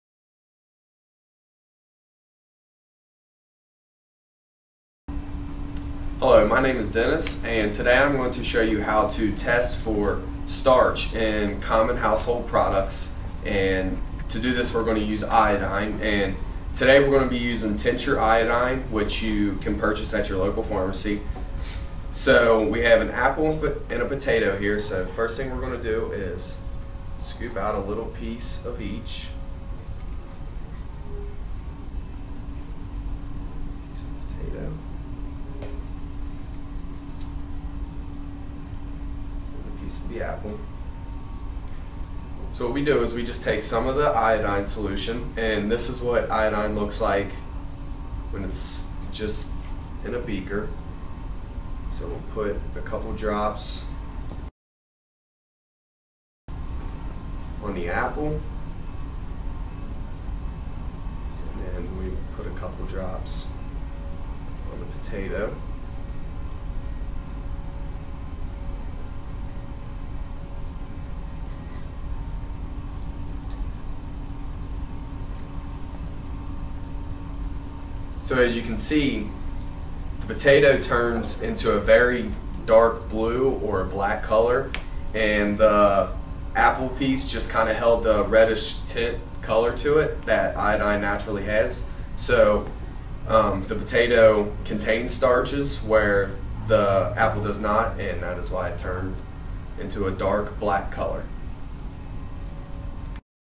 Starch Test
Testing for starch using an iodine solution - For more information including instructions, discussion of concepts, and other demonstrations visit the NKU demonstration database at ChemDemos.nku.edu